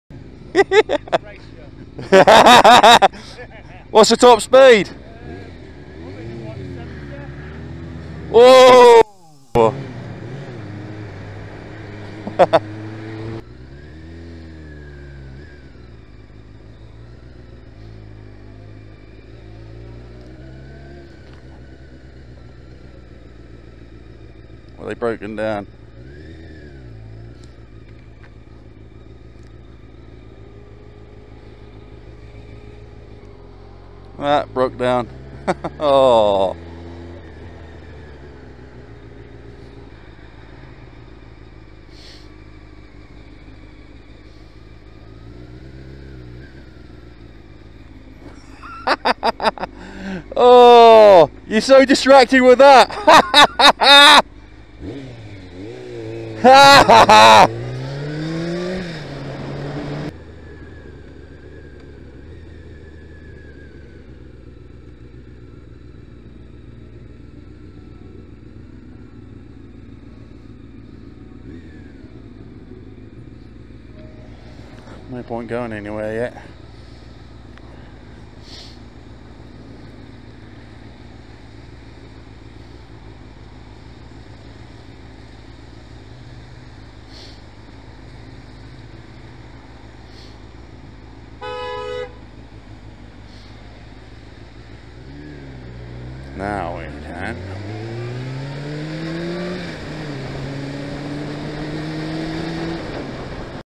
December Obs 11/12/21
Evening viewers, a few new clips for ya all to have a watch through. In this week's episode, we have junction blockers, phone users and some total planks.

Hope ya all enjoy the video, peace out and stay frosty